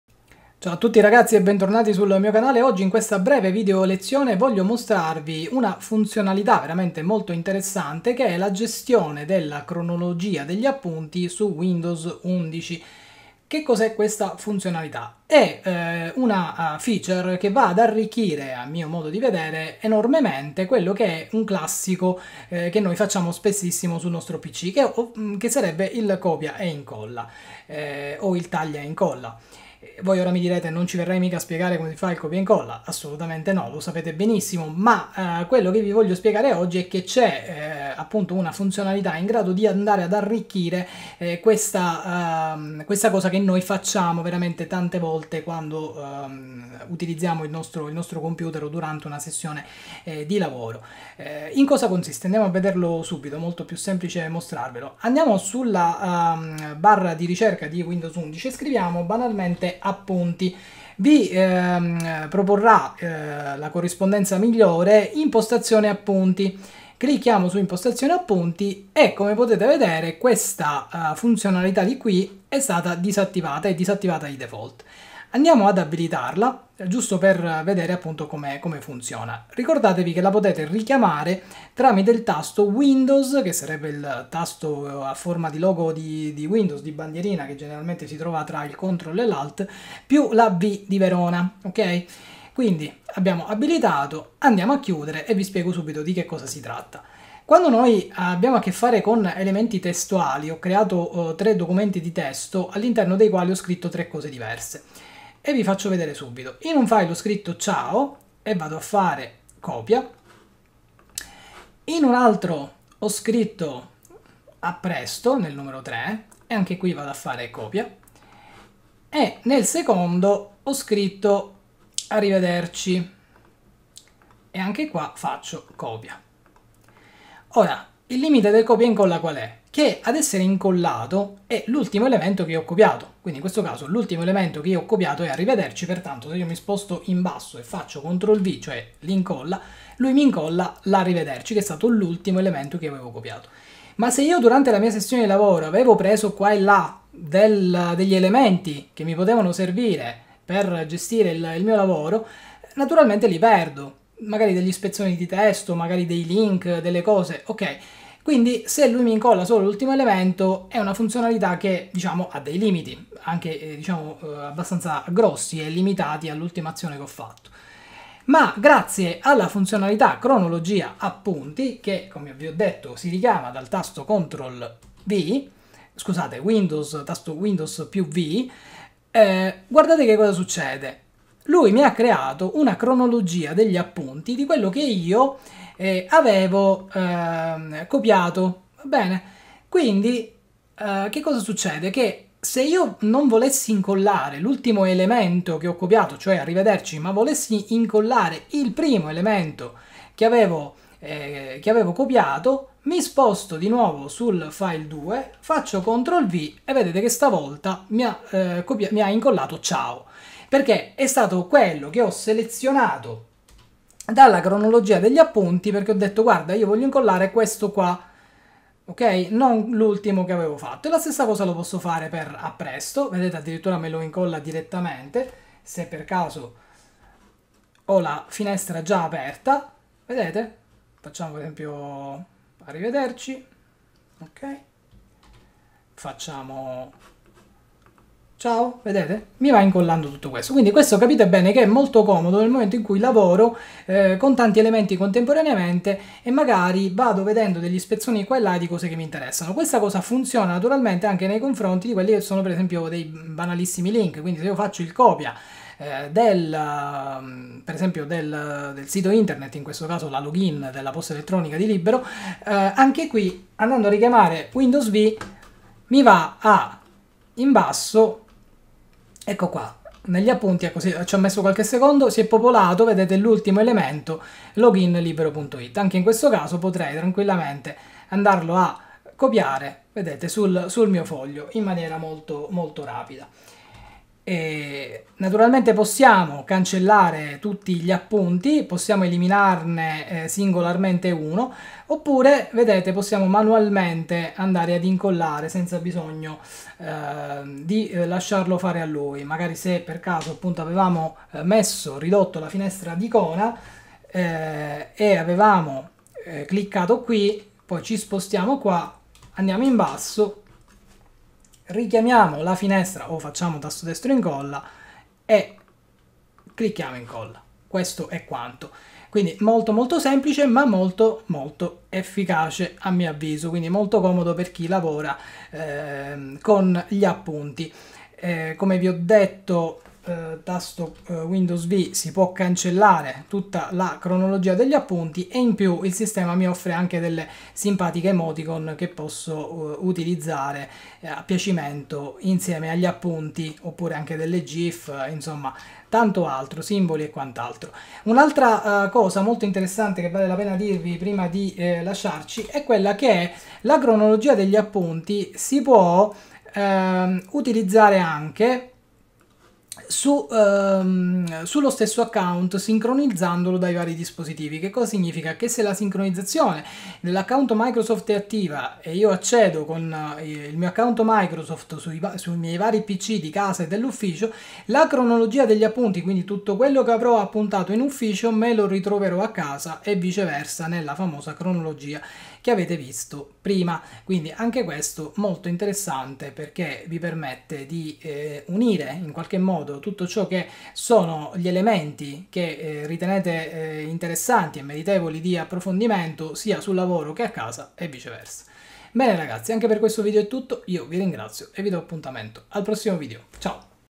Appunti di windows , come utilizzarli
Arricchisci il copia e incolla con la cronologia appunti